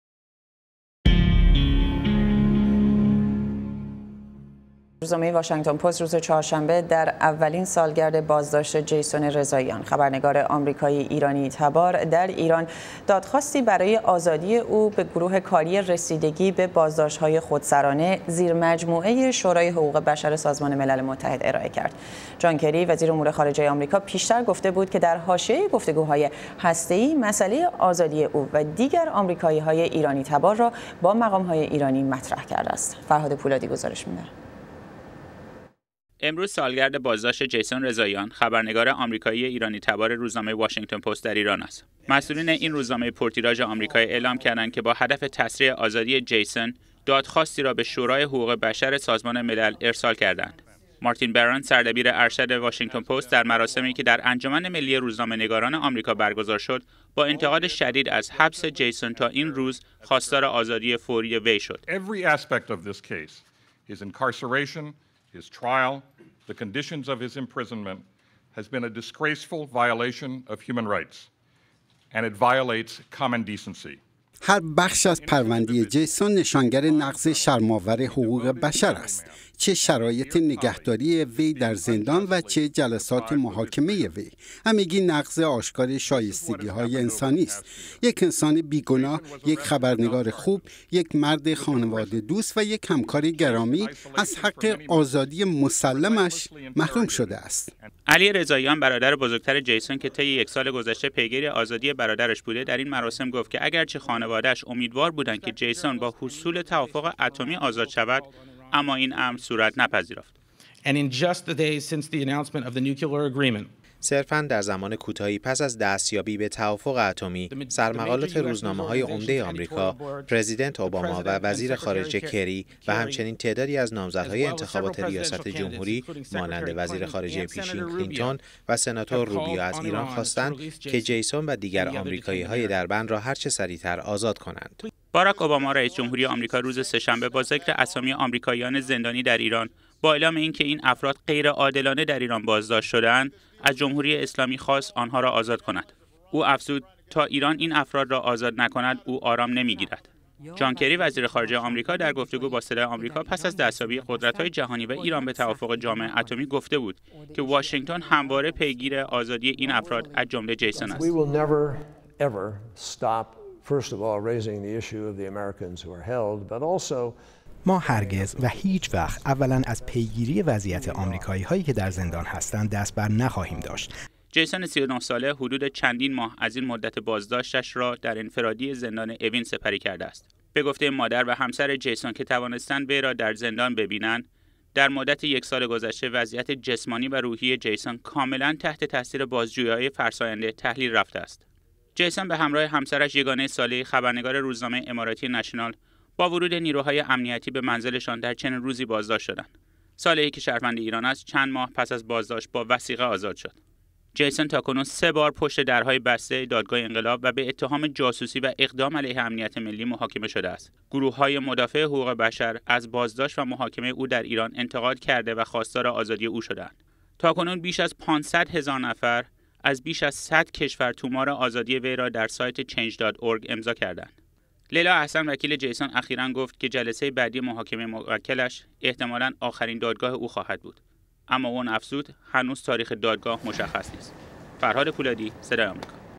سالگرد بازداشت جیسون رضائیان
روز چهارشنبه سالگرد بازداشت جیسون رضائیان خبرنگار آمریکایی - ایرانی تبار روزنامه واشنگتن پست در ایران است.گزارش فرهاد پولادی